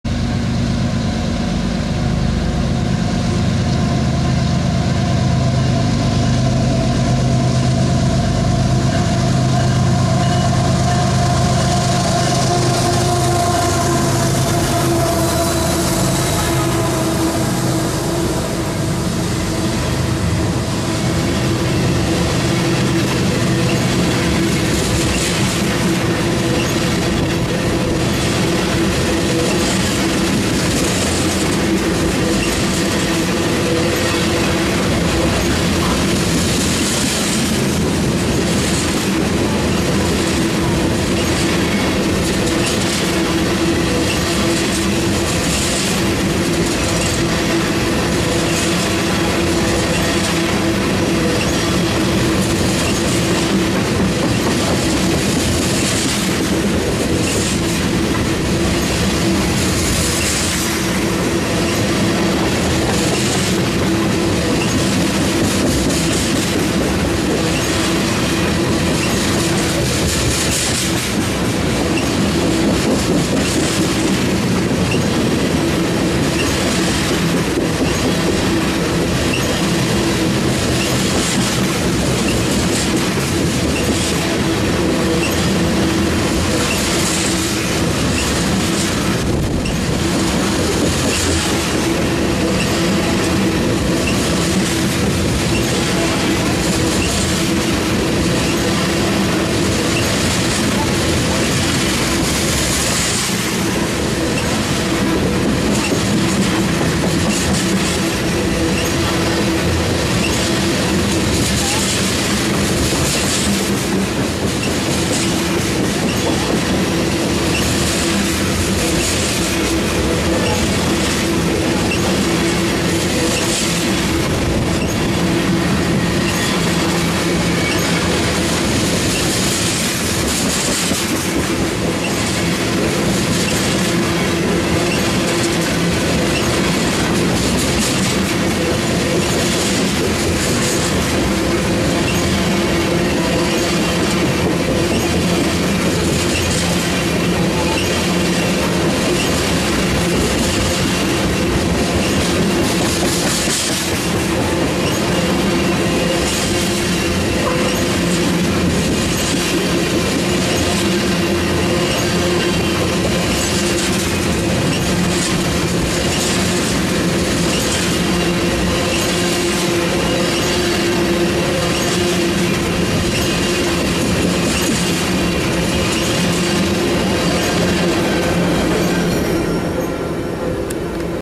CSX 817 With BNSF Power Leads Oil Train, Naperville, IL
CSX and BNSF Gevo's lead an eastbound oil train on the Chicago sub.